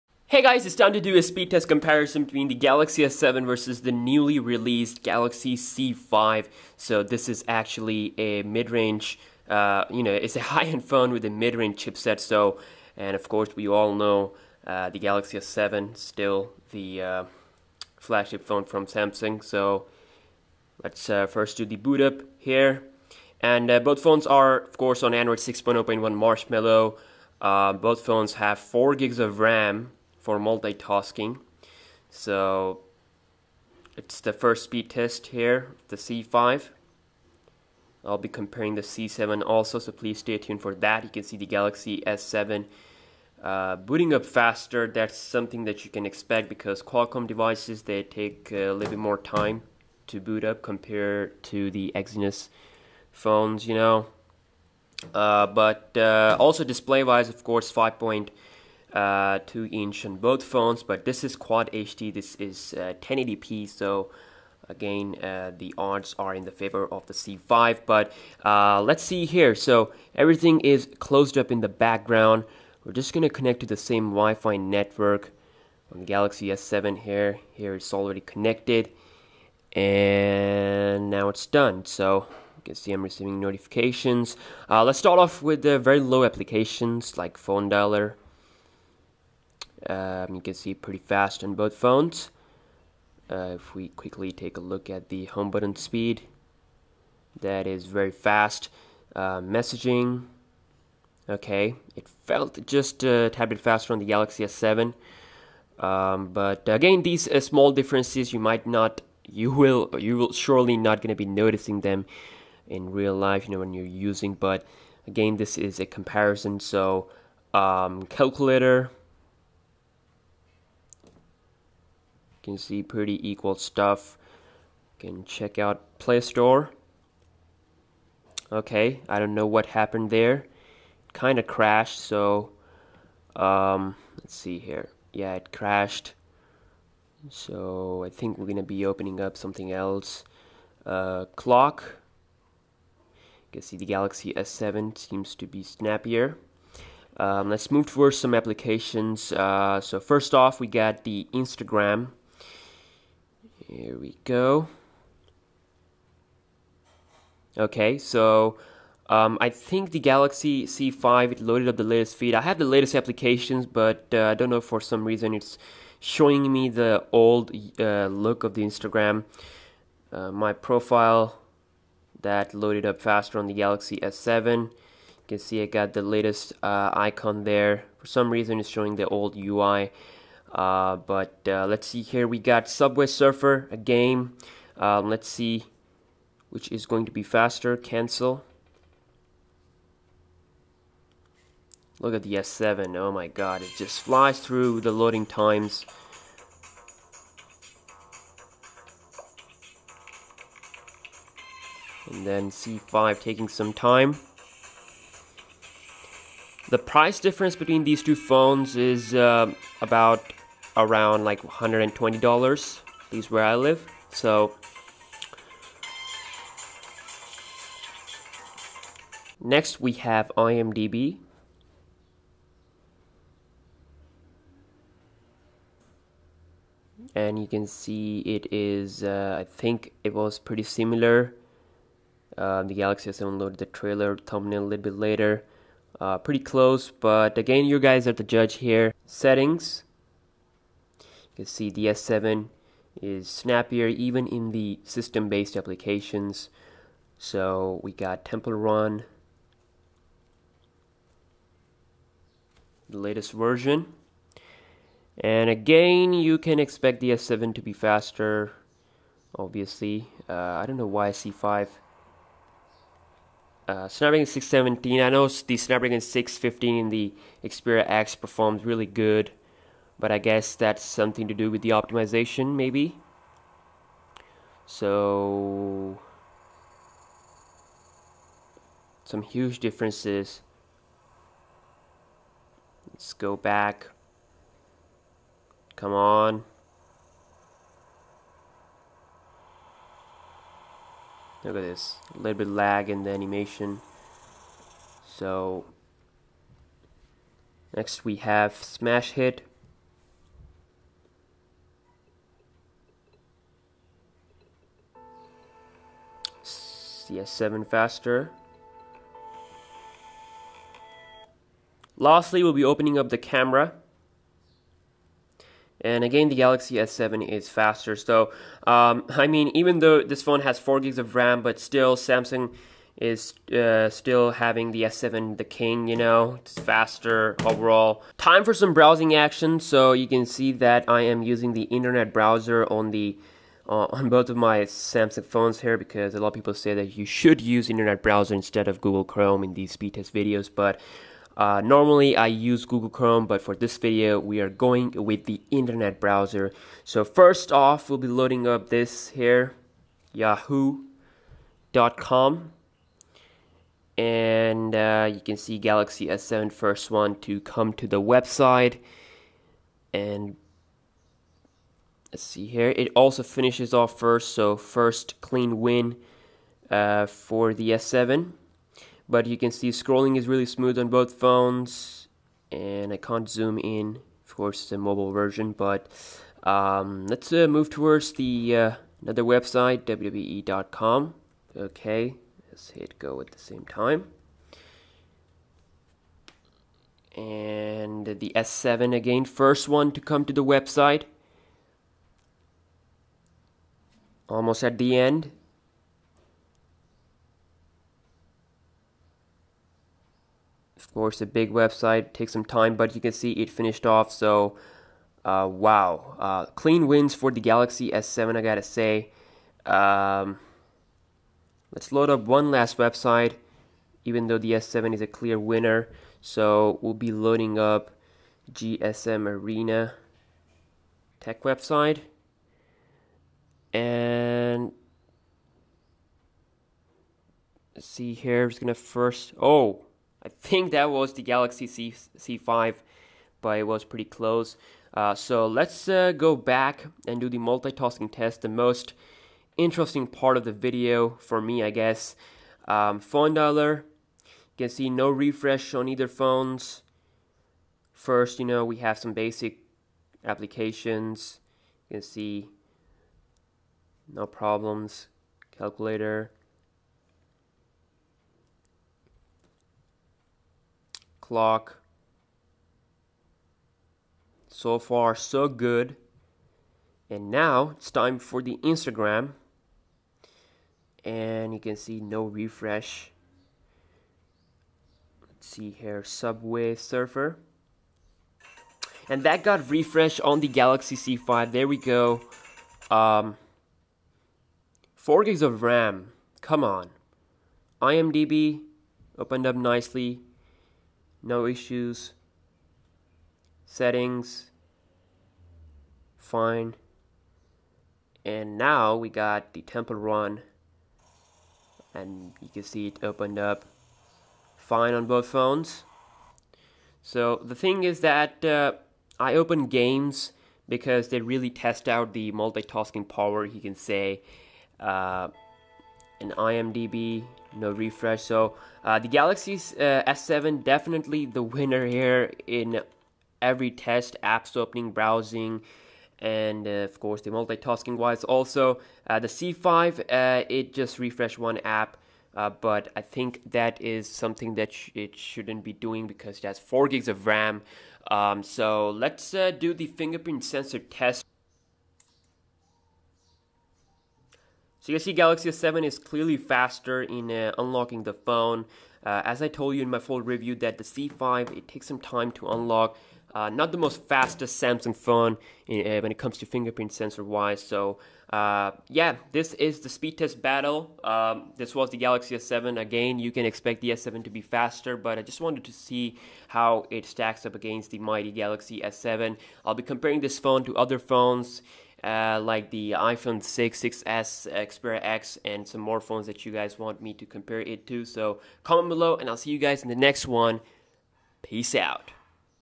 Samsung Galaxy C5 vs Galaxy S7 - Speed Test! (4K)
Samsung Galaxy C5 vs Galaxy S7 speed test comparison including apps opening, browsing, multitasking, booting up and fingerprint reader speed test.